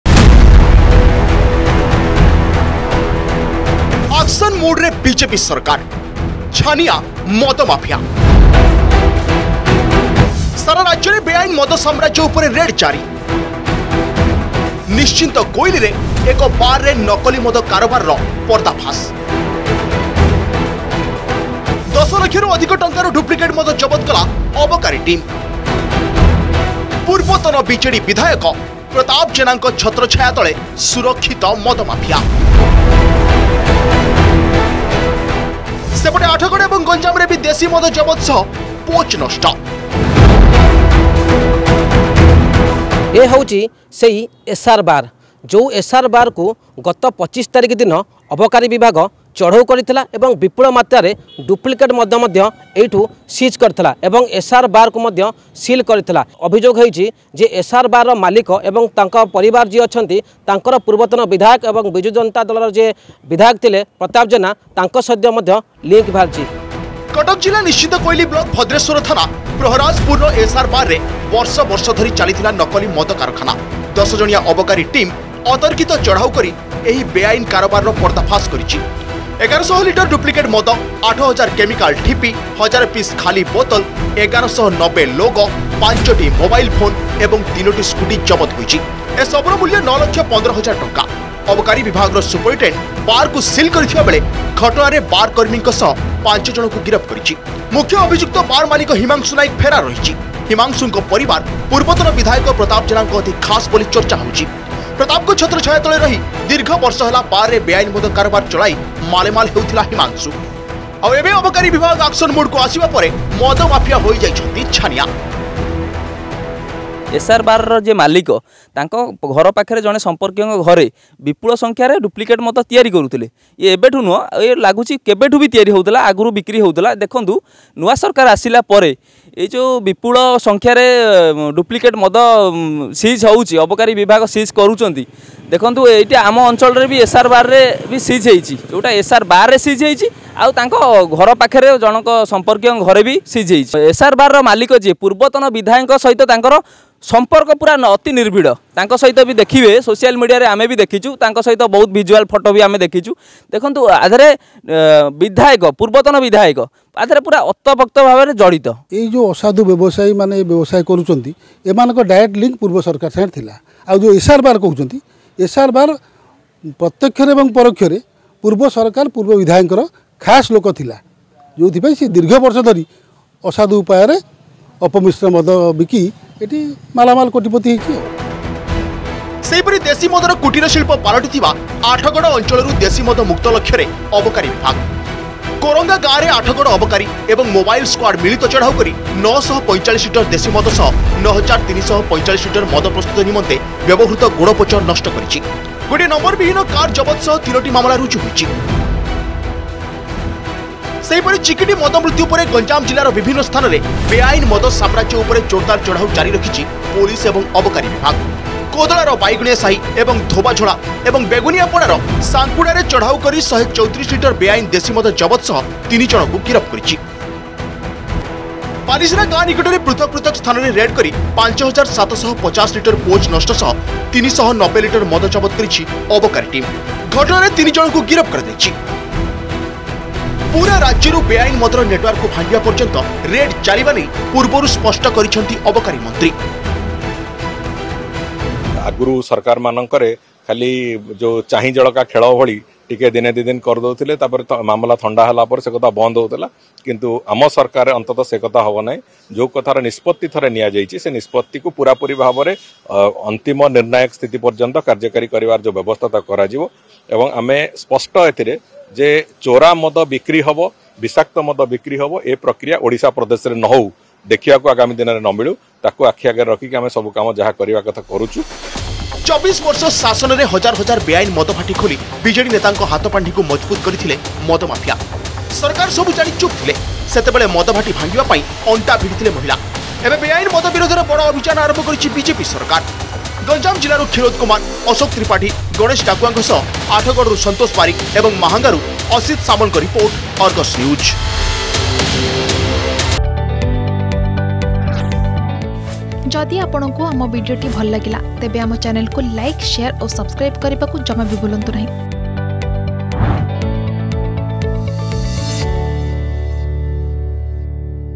ଆକ୍ସନ୍ ମୁଡରେ ବିଜେପି ସରକାର, ଛାନିଆ ମଦ ମାଫିଆ | Counterfeit wine | Excise Department | Argus News
ସାରା ରାଜ୍ୟରେ ବେଆଇନ୍ ମଦ ସାମ୍ରାଜ୍ୟ ଉପରେ ରେଡ୍ ଜାରି । ନିଶ୍ଚିନ୍ତକୋଇଲିରେ ଏକ ବାରରେ ନକଲି ମଦ କାରବାରର ପର୍ଦ୍ଦାଫାସ୍ । 10 ଲକ୍ଷରୁ ଅଧିକ ଟଙ୍କାର ଡୁପ୍ଲିକେଟ୍ ମଦ ଜବତ କଲା ଅବକାରୀ ଟିମ୍